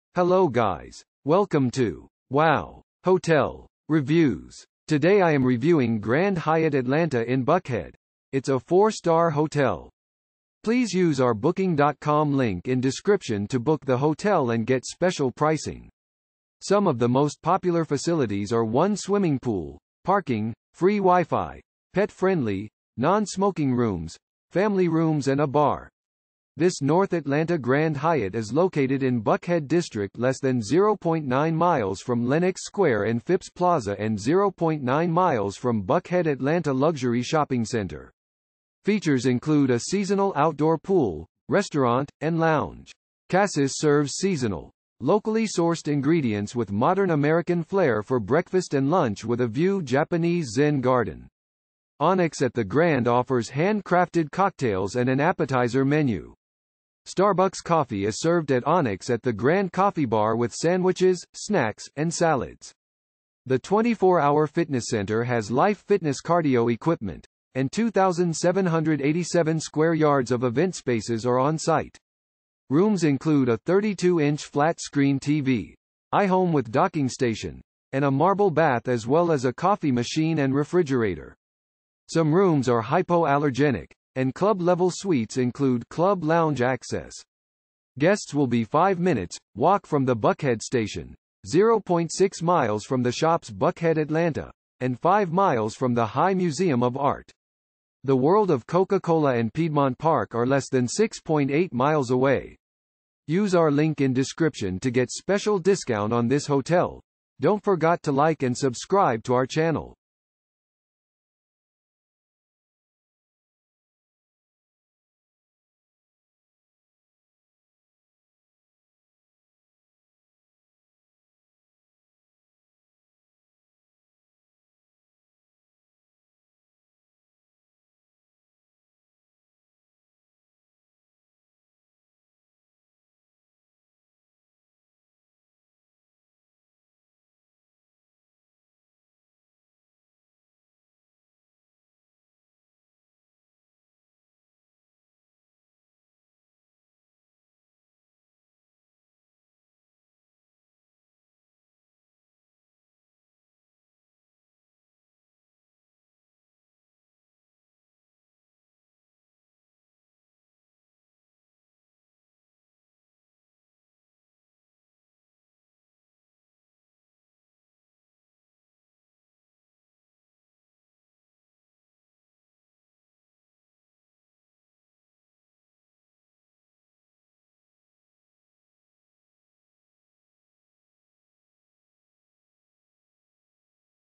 Grand Hyatt Atlanta in Buckhead Review - Atlanta , United States of America
Grand Hyatt Atlanta in Buckhead

This North Atlanta Grand Hyatt is located in Buckhead District less than 0.9 mi from Lenox Square and Phipps Plaza and 0.9 mi from Buckhead Atlanta luxury shopping center. Features include a seasonal outdoor pool, restaurant, and lounge.
Cassis serves seasonal, locally sourced ingredients with modern American flare for breakfast and lunch with a view Japanese Zen garden. Onyx at the Grand offers hand-crafted cocktails and an appetizer menu.
Starbucks Coffee is served at Onyx at the Grand Coffee Bar with sandwiches, snacks, and salads. The 24-hour fitness center has Life Fitness cardio equipment, and 2,787 square yards of event spaces are on site.
Rooms include a 32-inch flat-screen TV, iHome with docking station, and a marble bath as well as a coffee machine and refrigerator. Some rooms are hypo-allergenic, and club-level suites include club lounge access.
Guests will be 5 minutes’ walk from the Buckhead Station, 0.6 mi from The Shops Buckhead Atlanta, and 5 mi from the High Museum of Art. The World of Coca-Cola and Piedmont Park are less than 6.8 mi away.